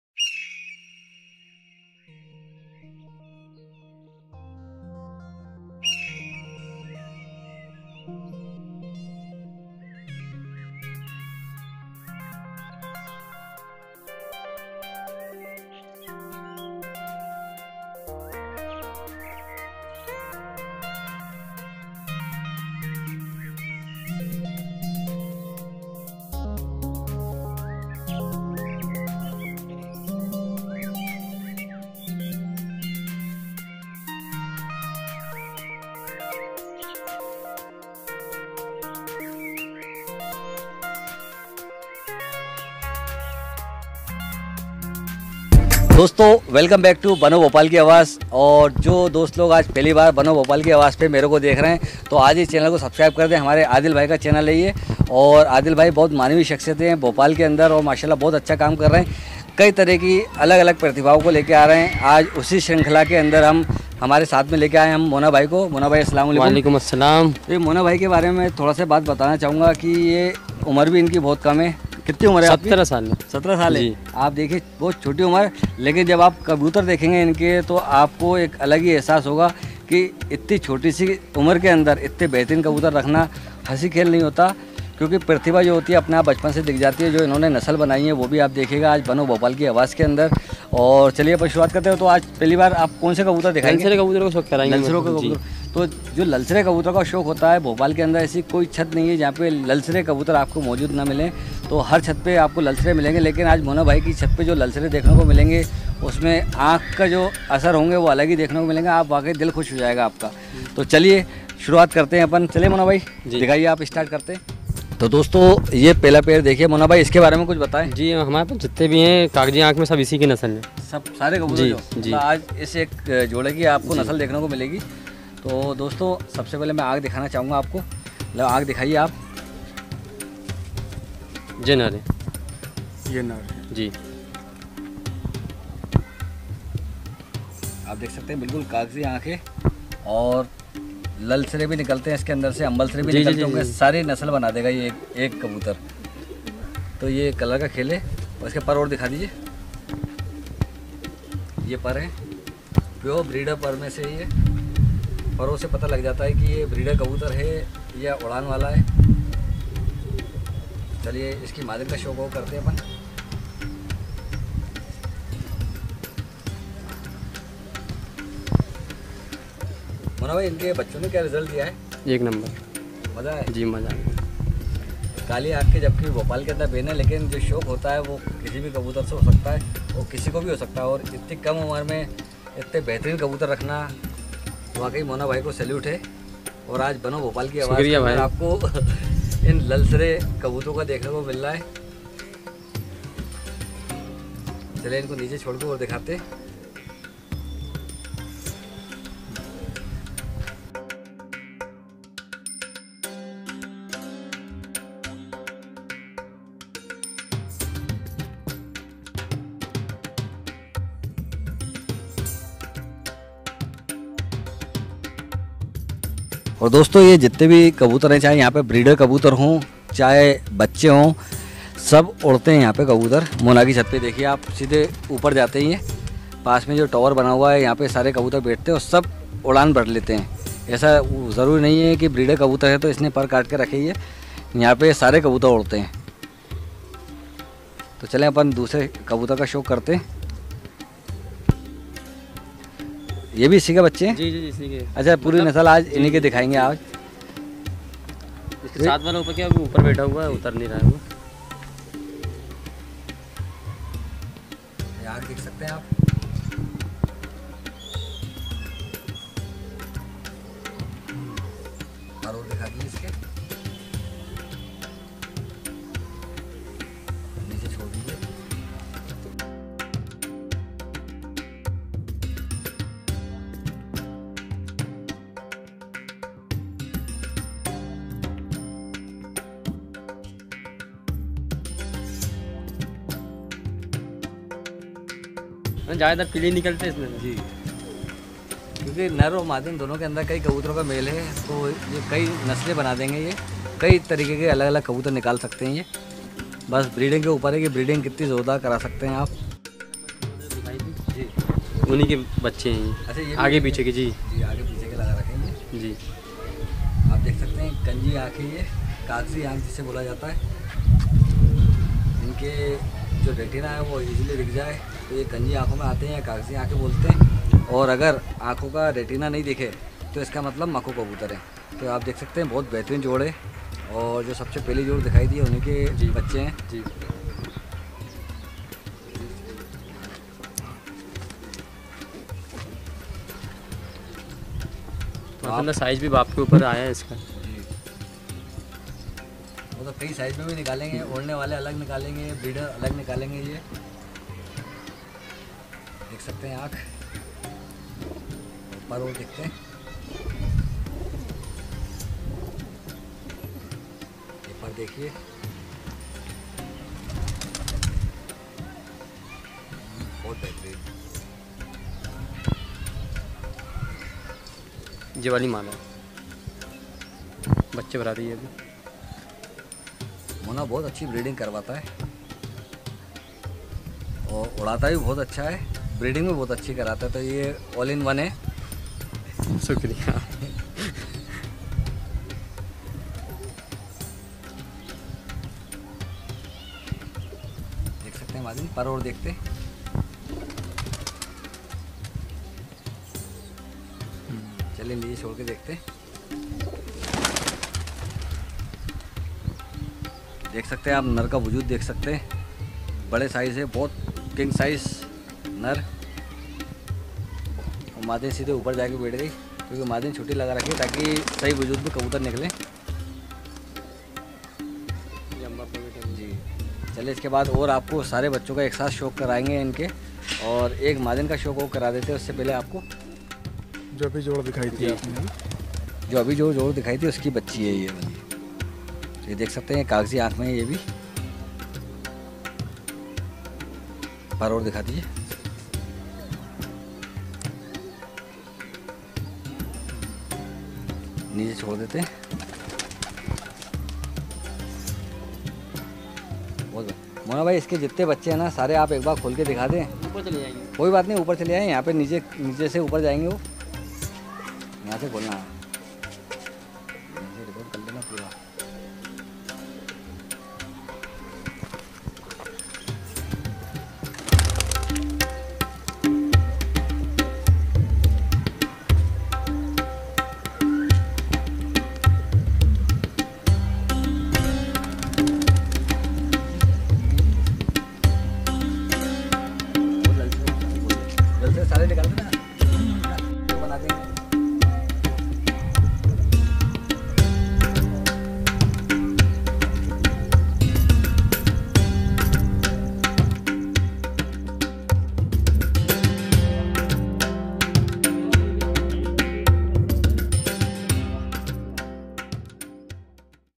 Mona Bhai Ke Lajawab Lalsire Kabutar
कबूतर बाजी की बहुत अच्छी जानकारी दी उस्ताद अब्दुल अलीम खान साहब(पप्पू कासनी) ने

उस्ताद अब्दुल अलीम खान साहब(पप्पू कासनी)

(पप्पू कासनी)

उस्ताद आग़ा अब्दुल अलीम खान।
 kabutar
Kabootar
Kabootar bazi
bhopal
Ustad Agha Abdul Aleem Khan